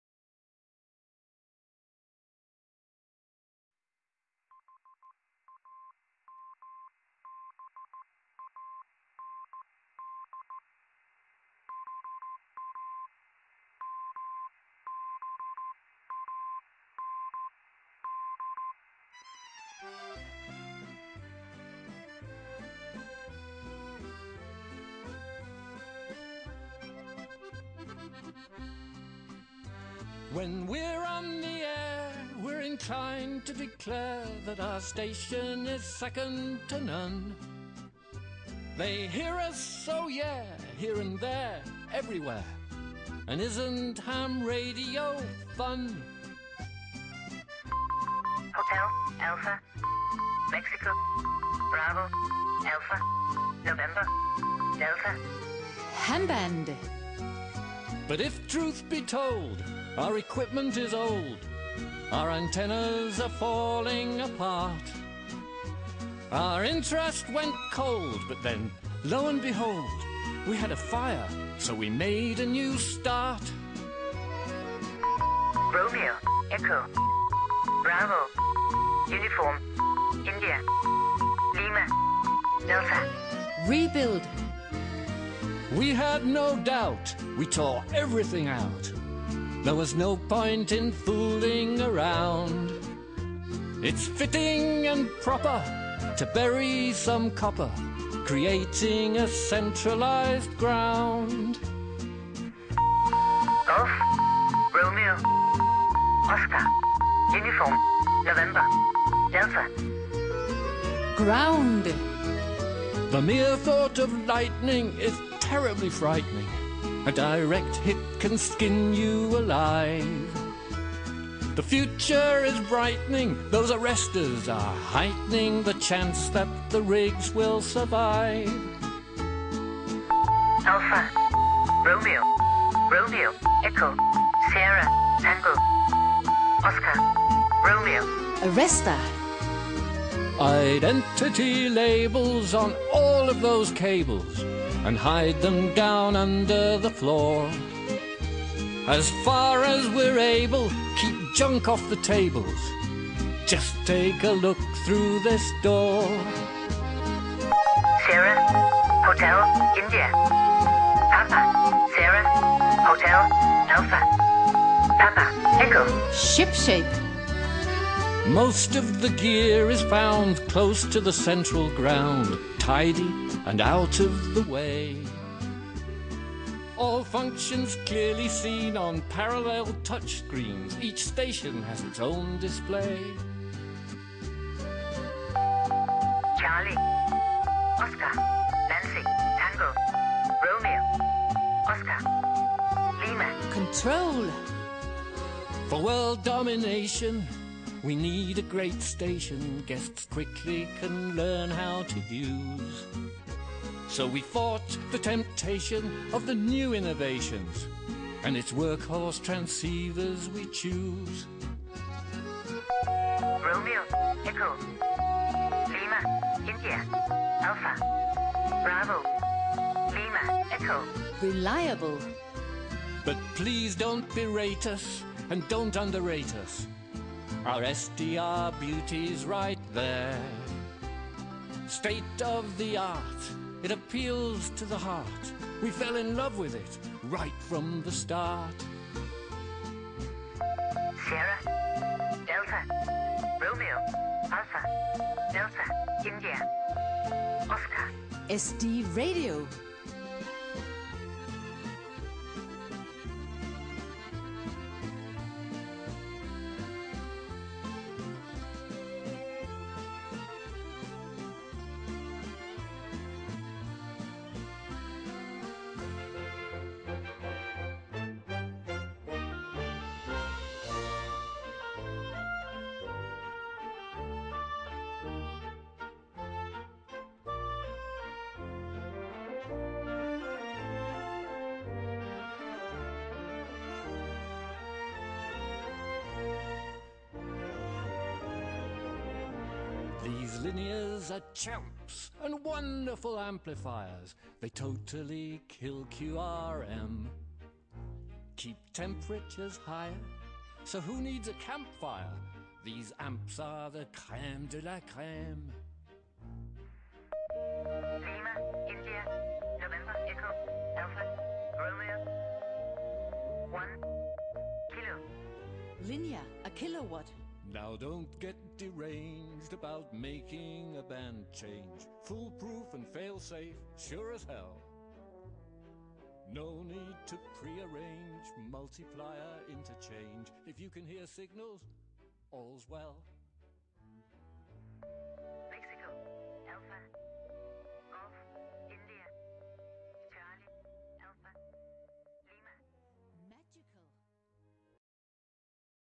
HAMBAND
a modern amateur radio station and at the same time enables the listener/viewer to learn Morse code.